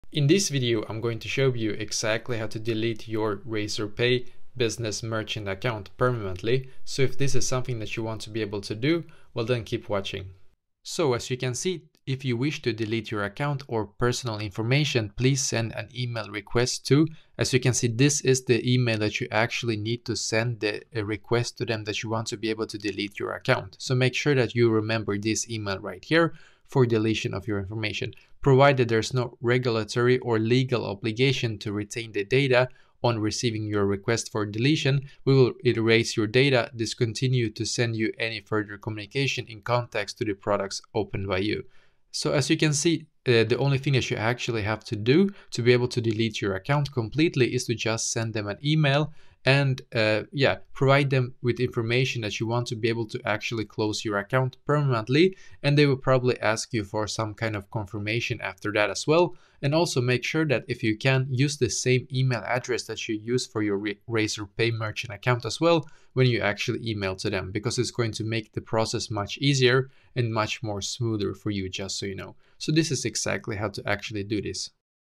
How To Delete My Razorpay Business Merchant Account Permanently (2025)
If you want to be able to delete your Razorpay business merchant account permanently, then this video will be perfect for you!